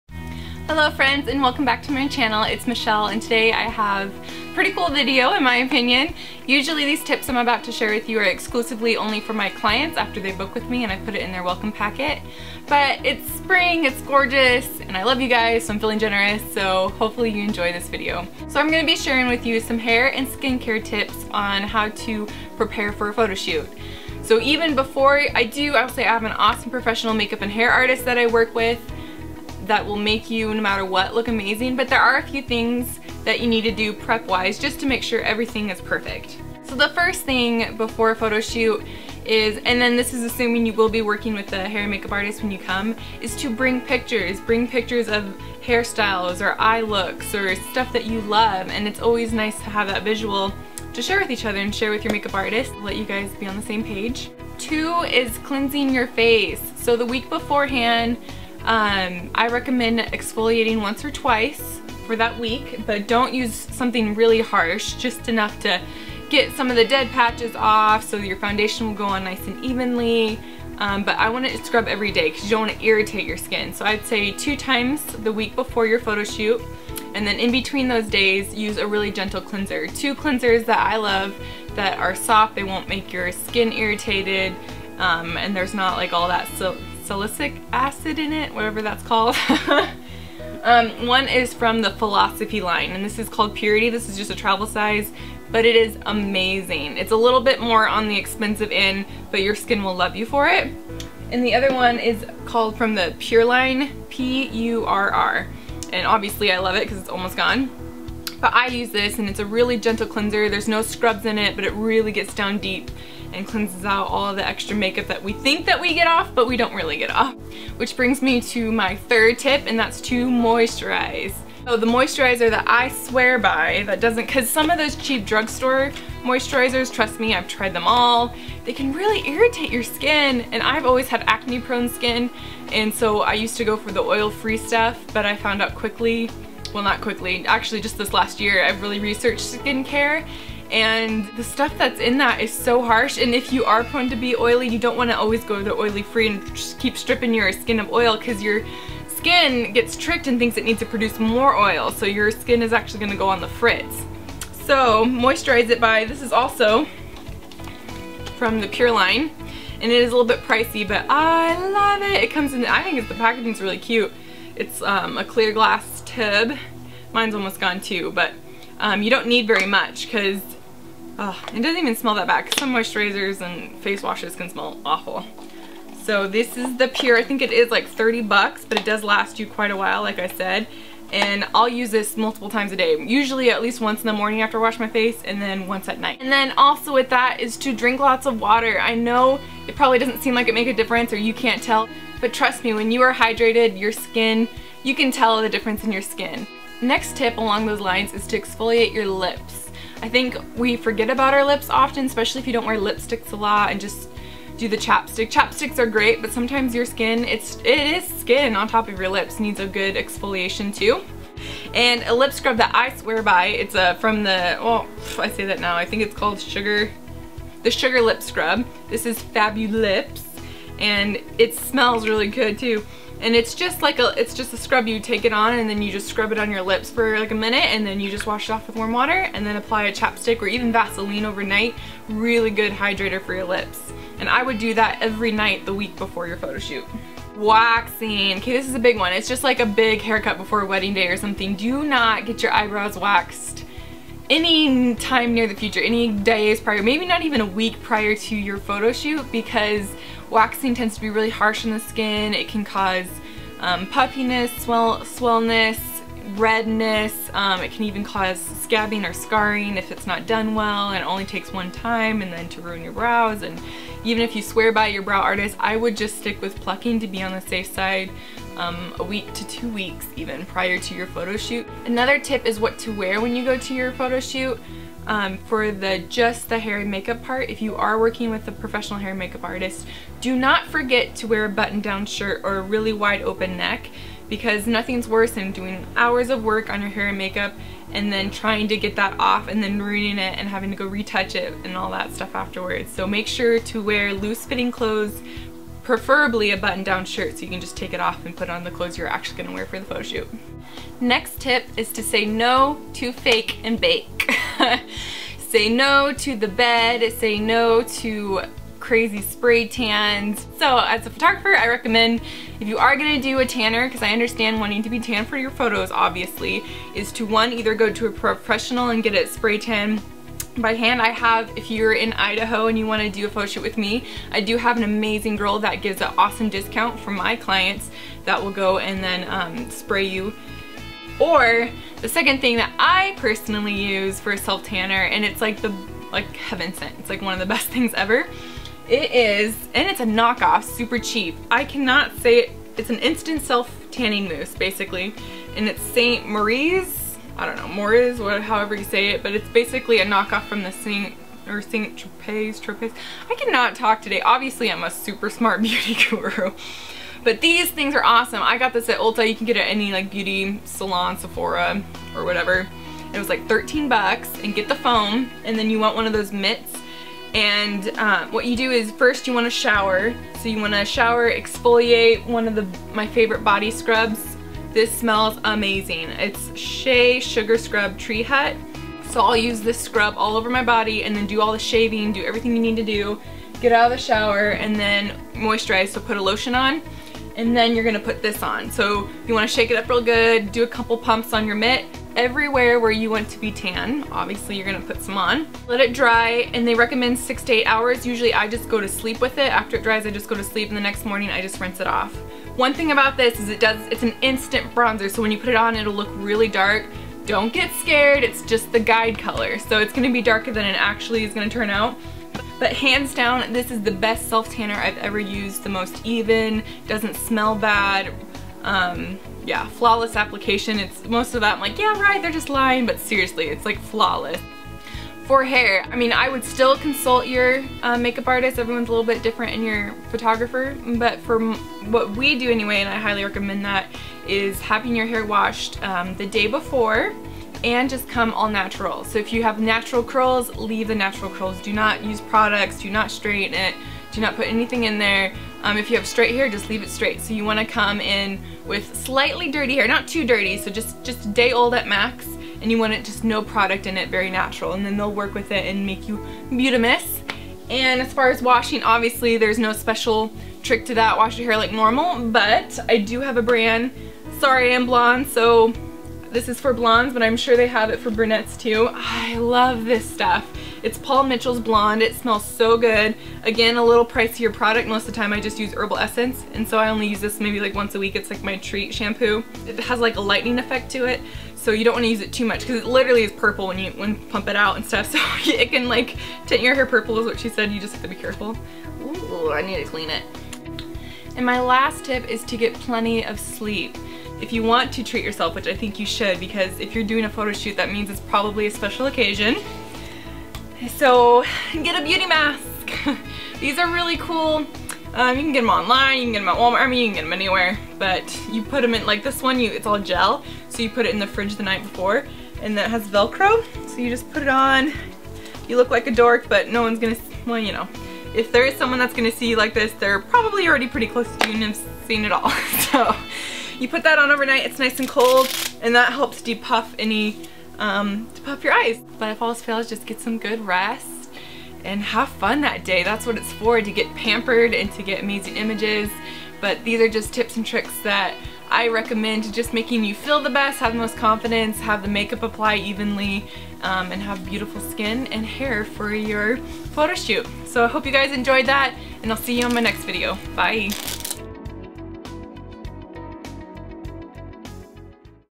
Skincare Tips Before A Photoshoot
I'm sharing some of my most exclusive tips on how to prepare your hair and skin before a photoshoot to make sure you are looking and feeling your best!

Check out my blog to see the breakdown of all these products.